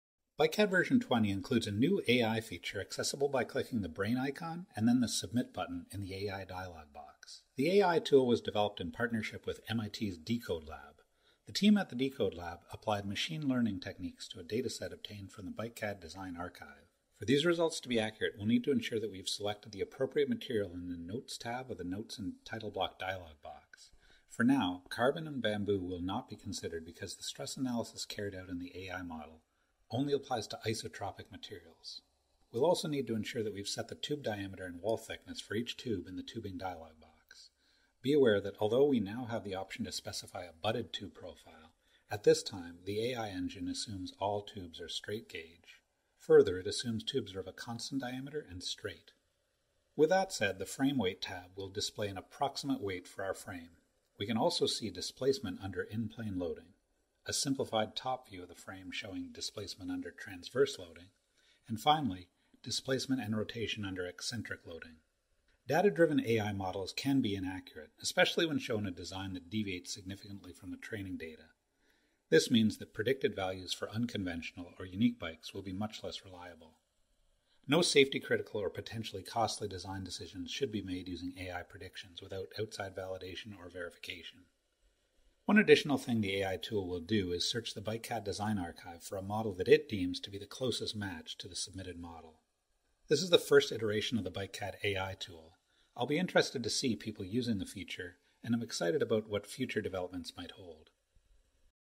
AI Feature in BikeCAD Pro
BikeCAD version 20 includes a new AI feature accessible by clicking the brain icon and then the submit button in the AI dialog box.

The team at the DeCoDE Lab applied machine learning techniques to a dataset obtained from the BikeCAD design archive. For these results to be accurate, we'll need to ensure that we have selected the appropriate material in the Notes tab of the Notes and title block dialog box. For now, carbon and bamboo will not be considered because the stress analysis carried out in the AI model only applies to isotropic materials. We'll also need to ensure that we've set the tube diameter and wall thickness of each tube in the Tubing dialog box. Be aware that although we now have the option to specify a butted tube profile, at this time, the AI engine assumes all tubes are straight gauge. Further, it assumes tubes are of a constant diameter and straight.

With that said, the Frame weight tab will display an approximate weight for our frame, we can also see displacement under in-plane loading, a simplified top view of the frame showing displacement under transverse loading and finally displacement and rotation under eccentric loading.

Data-driven AI models can be inaccurate, especially when shown a design that deviates significantly from the training data. This means that predicted values for unconventional or unique bikes will be much less reliable.

No safety-critical or potentially costly design decisions should be made using AI predictions without outside validation or verification.

One additional thing the AI tool will do is search the BikeCAD design archive for a model that it deems to be the closest match to the submitted model.

This is the first iteration of the BikeCAD AI tool. I'll be interested to see people using this feature and am excited about what future developments might hold.

Links to MIT's DeCoDE lab are listed below.